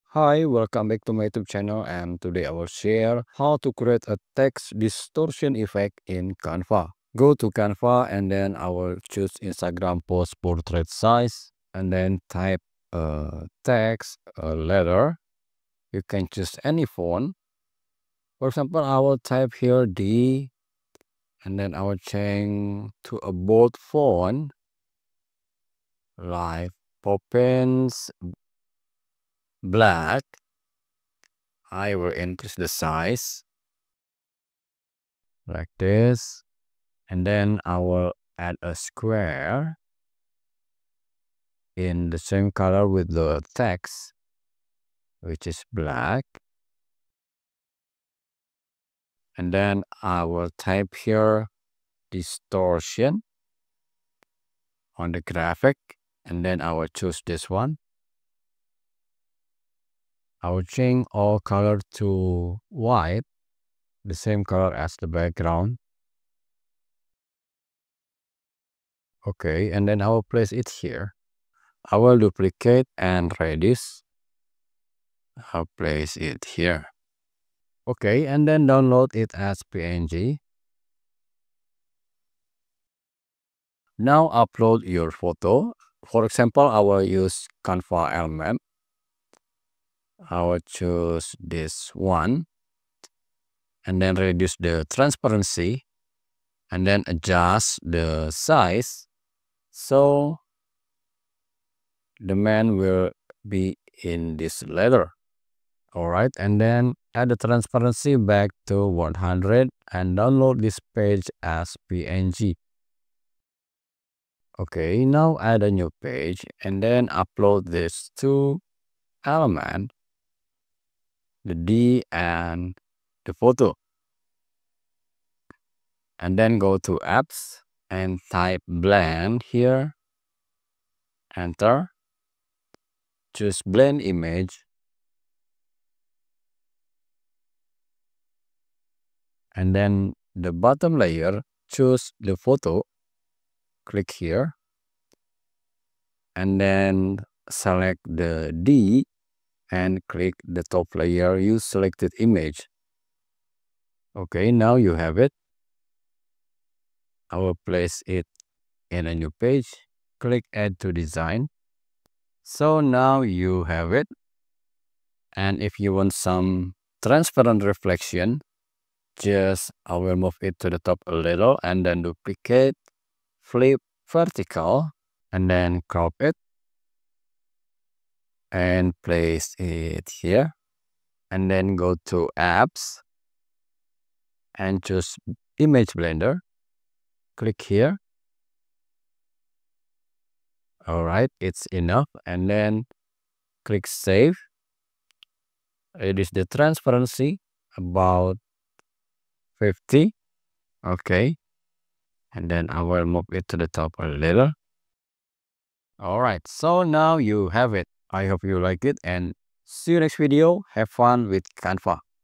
CREATE Distortion Text Effect for LETTER Portrait in Canva
Learn how to create a stunning distortion text effect for a letter portrait in Canva! In this
tutorial, we'll show you a step-by-step guide on how to achieve this unique and
eye-catching effect using Canva's powerful design tools. Whether you're a graphic
designer, social media manager, or content creator, this effect is perfect for adding a
touch of creativity to your designs. Follow along and get ready to take your design skills
to the next level!
Do you want to make 3D mockup animation?